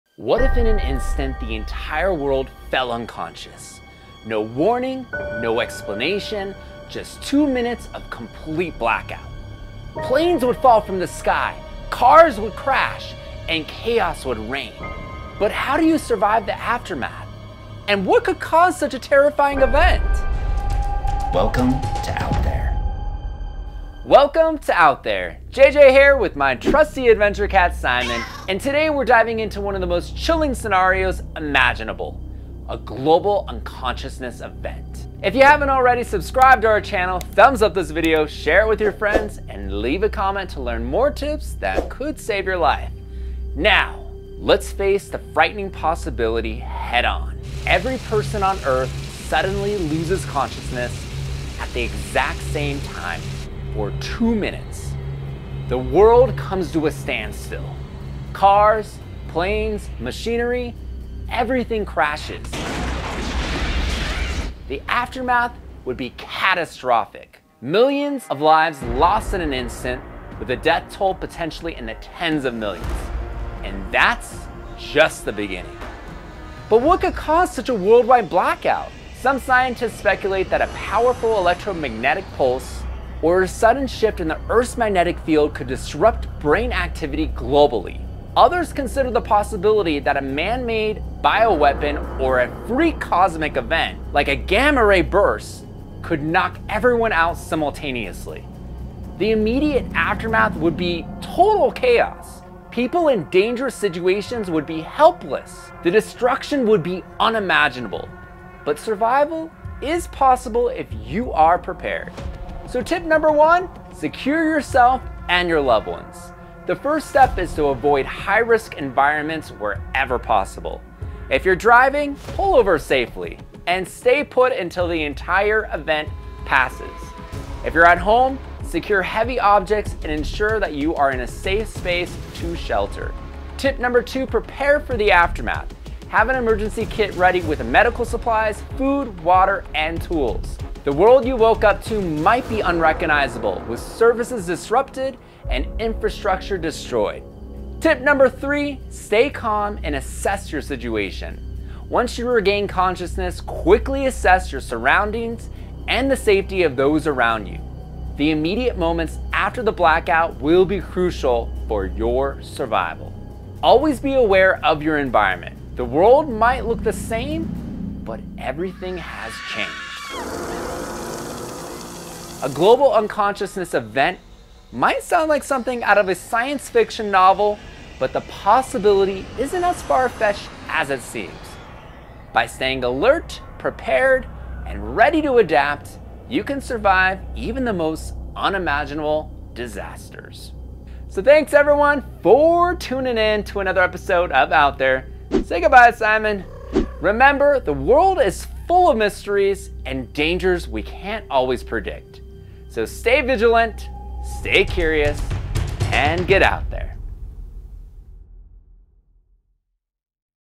What If the Entire World Went Unconscious for 60 seconds? Could you survive?
What If the Entire World Went Unconscious? 🌍 A Terrifying Survival Scenario!

Imagine this: In an instant, the entire world falls unconscious—every person, everywhere, for two full minutes. Planes crash, cars collide, and chaos consumes the world. What would you do? Could you survive the unimaginable aftermath of a global blackout?

In this gripping episode of OUT THERE, Simon and I dive into the chilling possibility of a Global Unconsciousness Event. From the catastrophic consequences to essential survival tips, we break down everything you need to know to stay alive.

Here’s what we’ll cover:
✔️ What Causes Global Unconsciousness?

EMPs or shifts in Earth’s magnetic field
Bioweapons or cosmic phenomena like gamma-ray bursts
✔️ The Immediate Aftermath:

Planes falling from the sky
Collapsed infrastructure and widespread destruction
✔️ How to Survive:

Tip 1: Secure yourself and avoid high-risk environments
Tip 2: Prepare an emergency kit with food, water, and medical supplies
Tip 3: Assess your surroundings and prioritize safety once conscious
💡 Did you know? A two-minute global blackout could result in millions of deaths and catastrophic infrastructure failures. The aftermath would leave the world unrecognizable.

Prepare for the Unthinkable:
Explore our Amazon Survival Gear for the tools you need to be ready for any disaster, even this one.